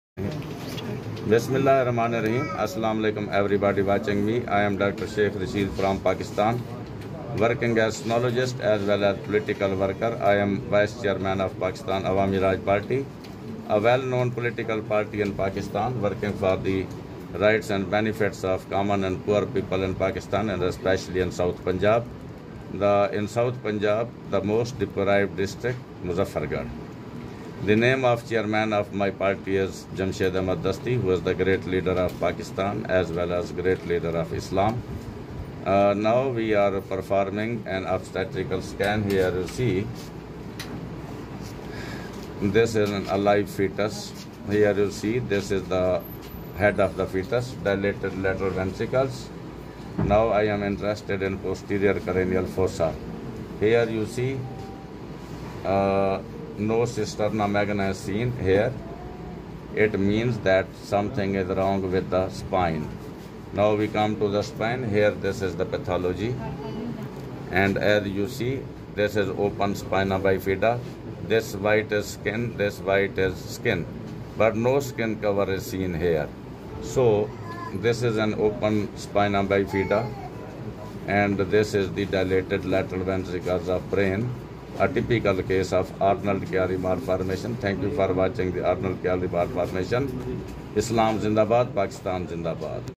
Arnold Chiari Malformation
Arnold Chiari Malformation ultrasound video by Dr,Abdul rasheed sheikh Pakistan